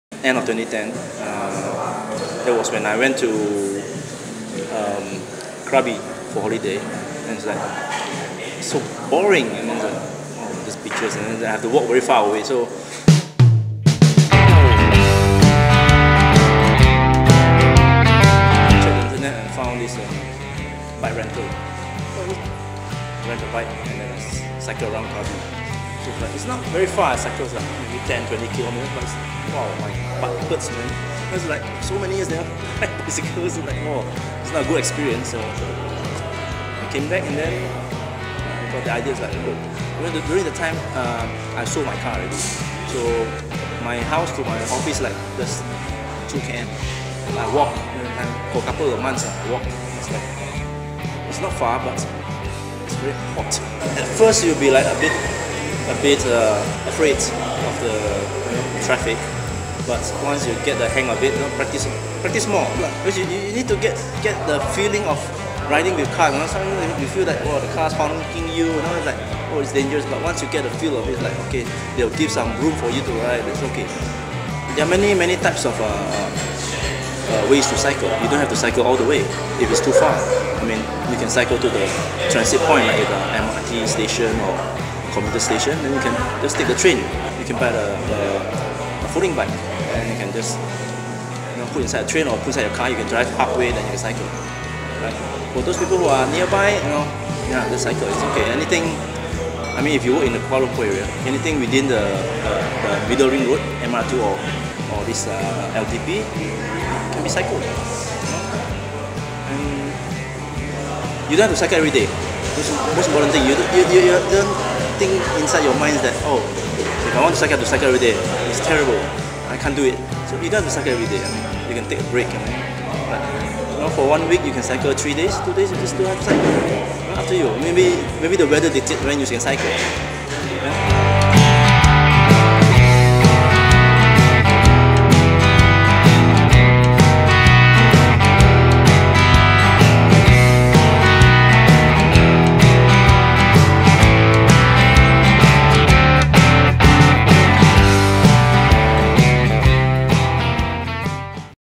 Chan Jer Ping - Cyling to Work
A holiday changed Jer Ping's choice of everyday transport.